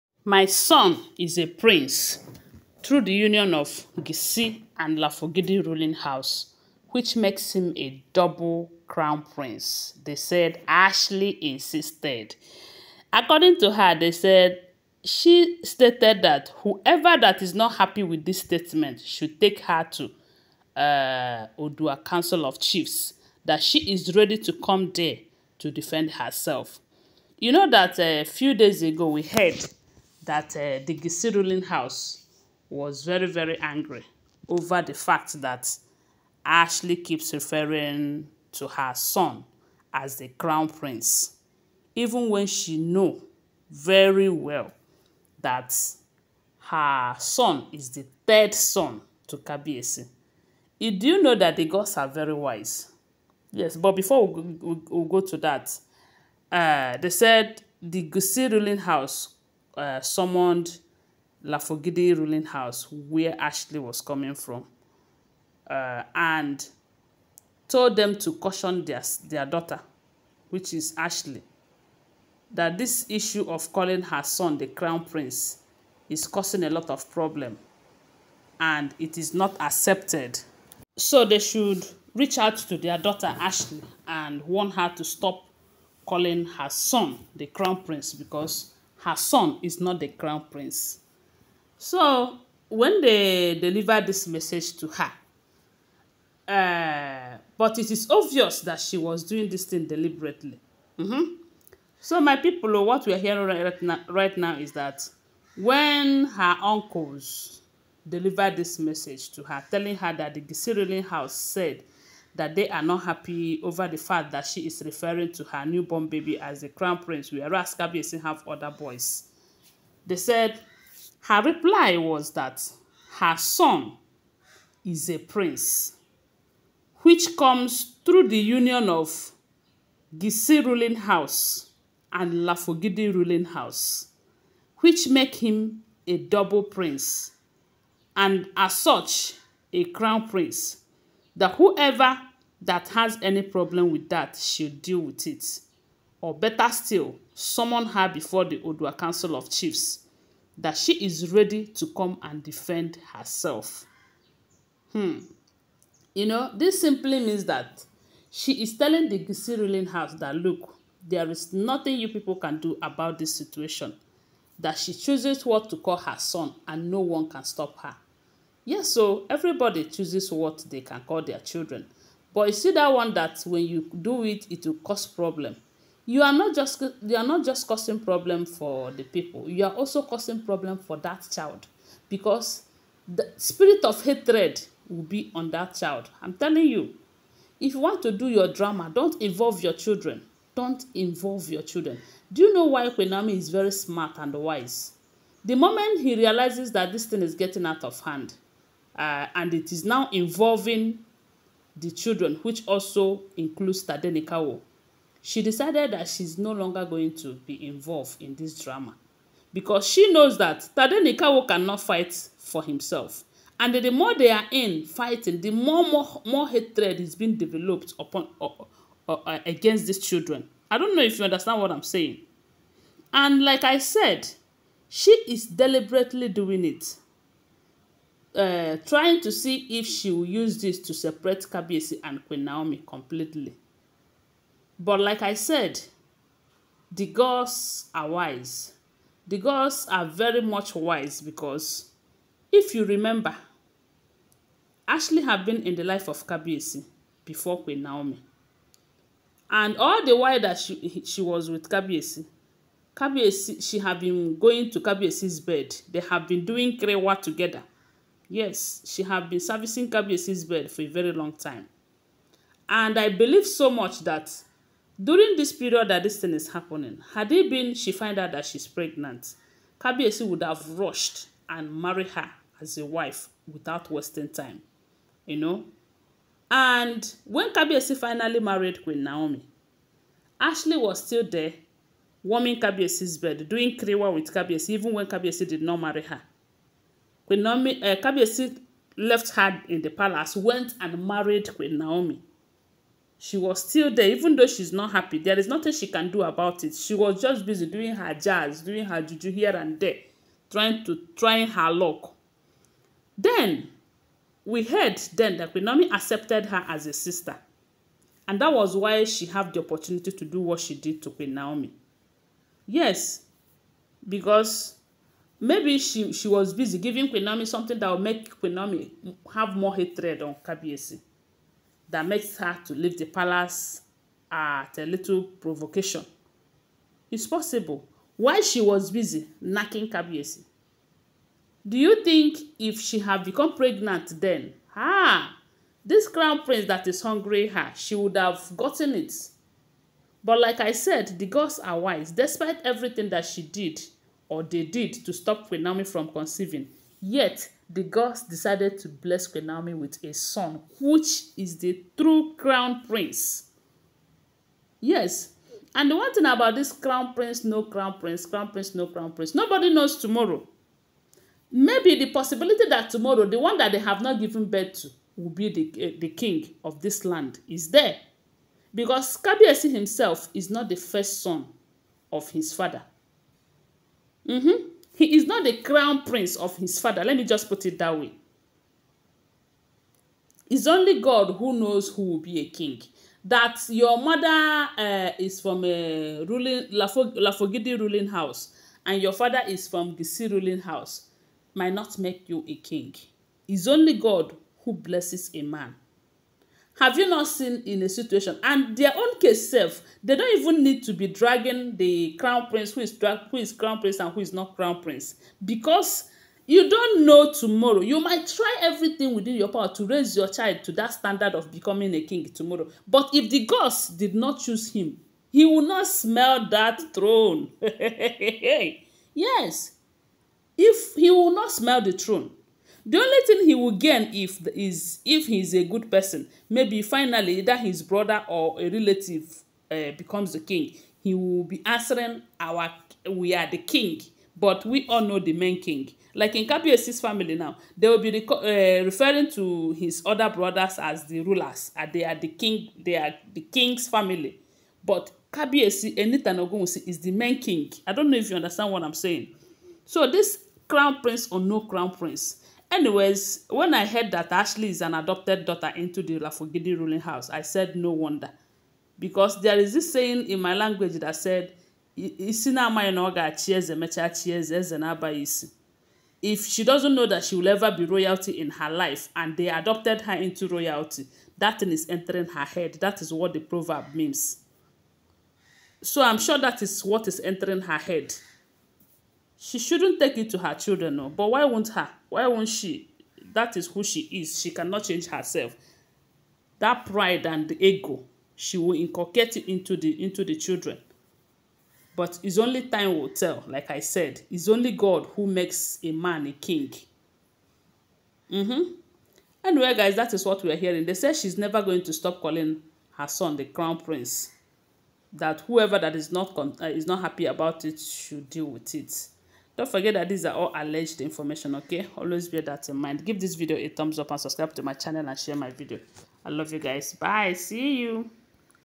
My Son Is a Prince from Gesi & Lafogidi House Which Makes Him A double/ Crown Prince Ashley Insisted
My Son Is a Prince Through the Union of
Gesi and Lafogidi Rulling House Which Makes Him A double/ Crown Prince Ashley Insisted.

Ooni of Ife's father commends Dr Elizabeth and Ooni of Ife for Re-